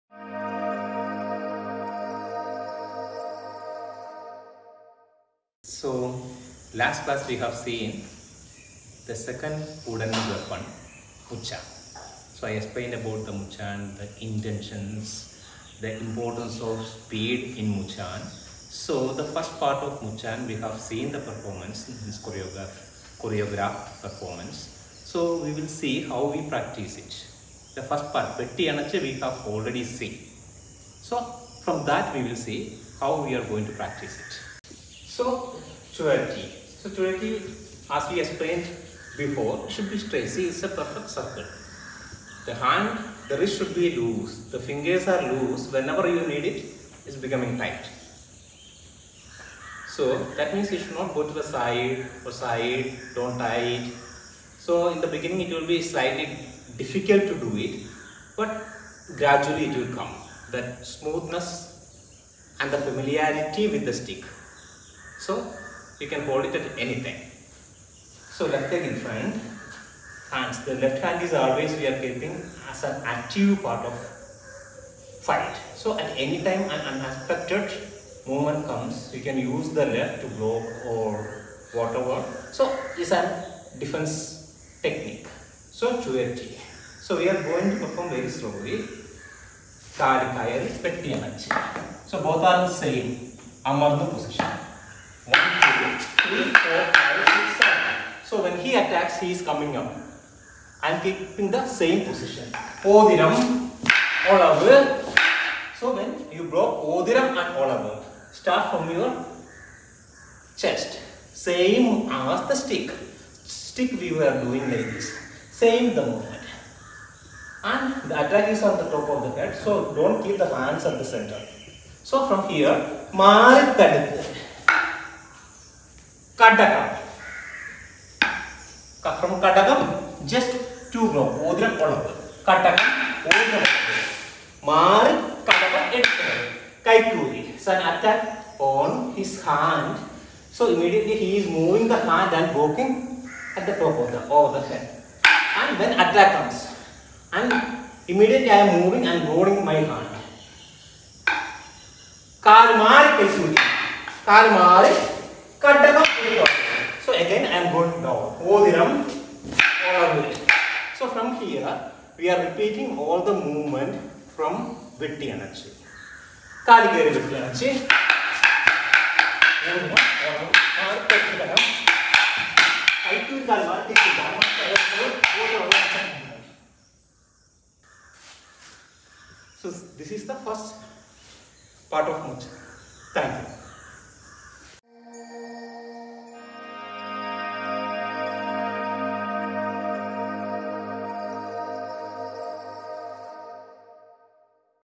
Muchaan [Part 1] | Explaination [2/2]
We complete the tutorial for the first sequence in Muchaan.